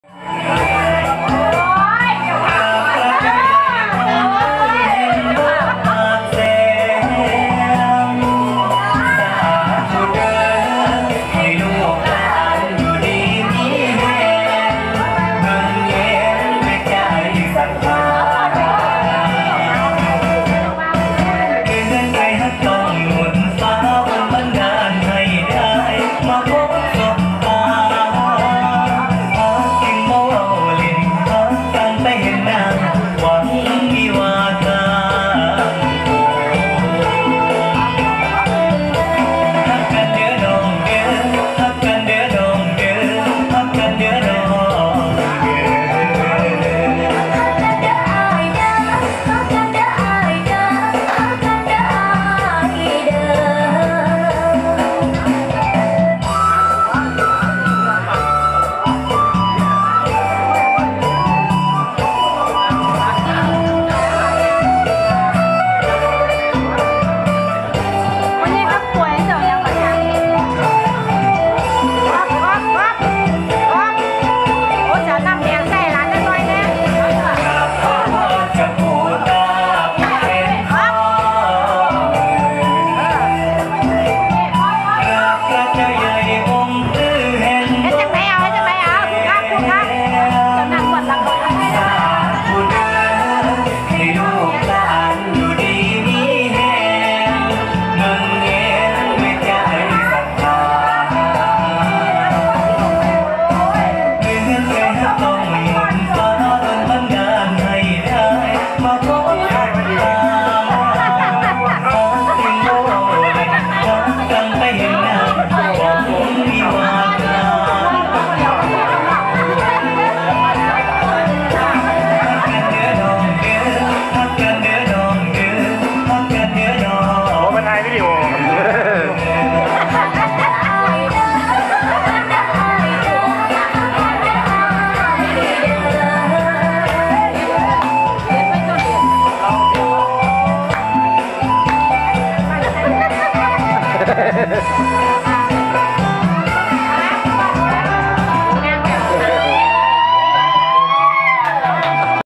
Gaun at Thoy's Wedding
A local wedding today in Si Bun Ruang brought out the natural Isaan dancing skills in my wife Gaun. It doesn't take much to get Isaan people "on the floor".